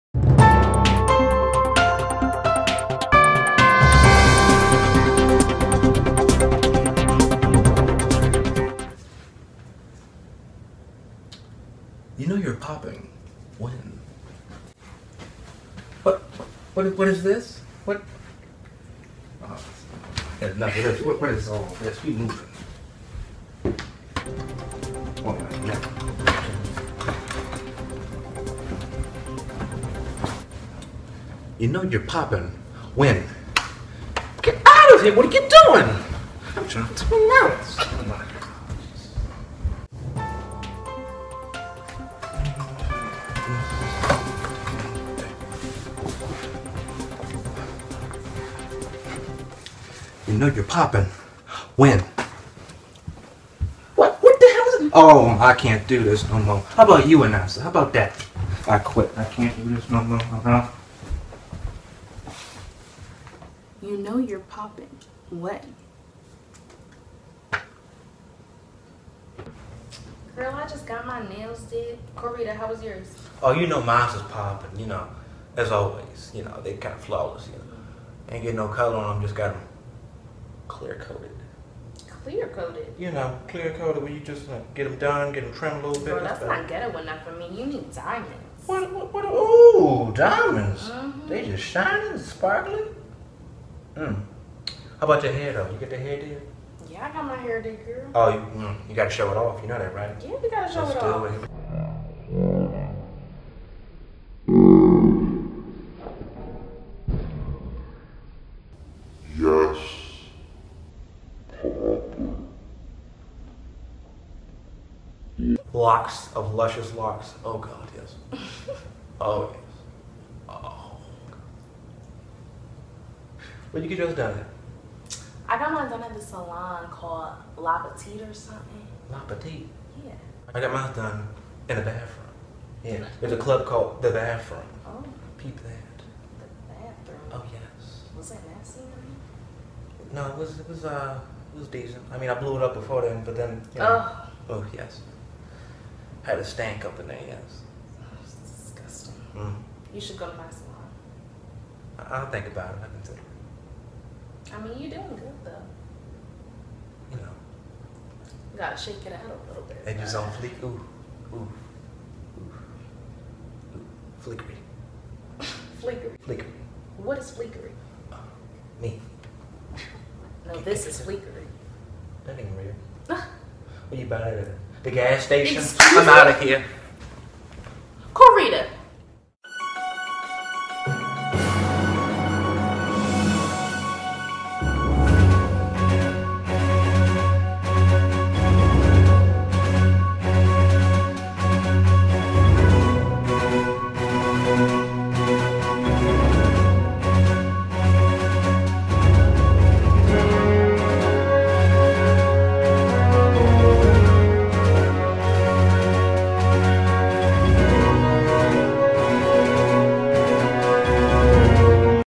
P.S.A. of the Day | You Know You're Poppin' When (EP5)
The announcer is disrupted by something very...poppin'.

Peep ya next time